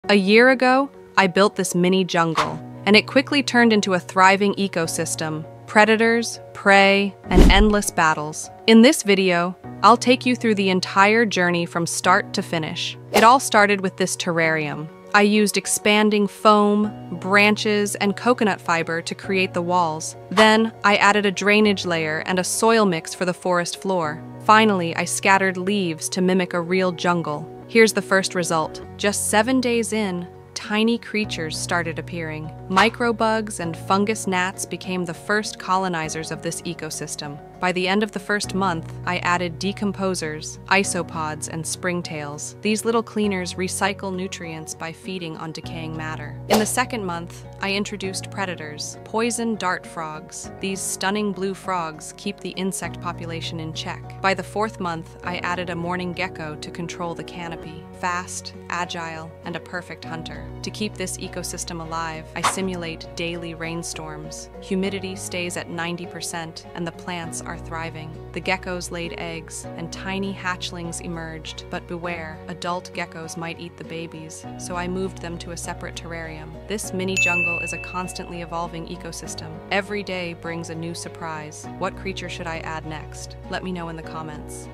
🌿 Creating a Mini Rainforest Ecosystem in 1 Year! 🐸🦎
One year ago, I started this mini rainforest project, and it transformed into a self-sustaining ecosystem! 🐜🦠 Predators, prey, decomposers, and a constantly evolving balance... In this video, I take you through this fascinating journey featuring poison dart frogs, cloning geckos, and so much more. 🌧️🌱

Building the terrarium and creating the forest floor 🌿

First inhabitants: Micro bugs and fungus gnats 🐞

Decomposers: Isopods and springtails 🦠

Predators: Poison dart frogs and morning geckos 🐸🦎

Rainforest simulation and plant growth 🌧️

Gecko eggs and the story of the hatchlings 🥚

This mini ecosystem shows just how incredible nature is! What creature should I add next? Let me know your ideas in the comments! 💬